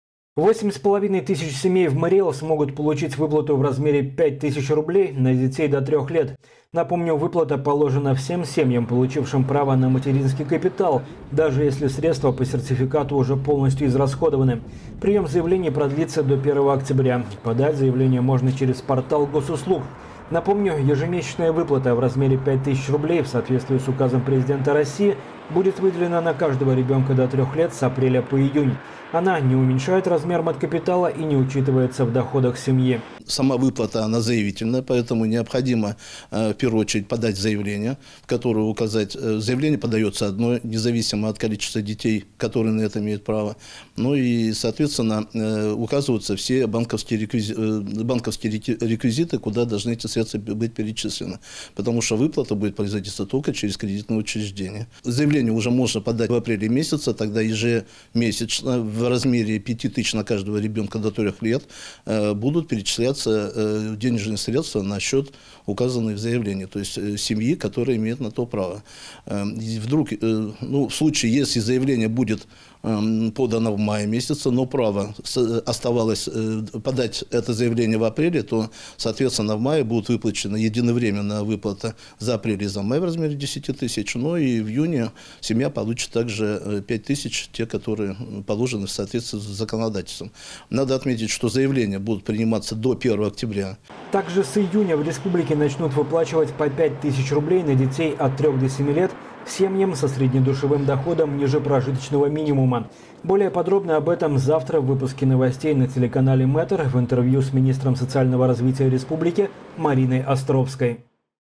8500 семей в Марий Эл смогут получить выплату в 5 тысяч рублей на детей до трех лет
Выплата положена всем семьям, получившим право на материнский капитал, даже если средства по сертификату уже полностью израсходованы. Прием заявлений продлится до 1 октября. Подать заявление можно через портал Госуслуг. Напомним, ежемесячная выплата в размере 5 тысяч рублей, в соответствии с Указом Президента России, будет выделена на каждого ребенка до 3-х лет с апреля по июнь. Она не уменьшает размер материнского капитала и не учитывается в доходах семьи.

Выпуск от 14 апреля 2020 г.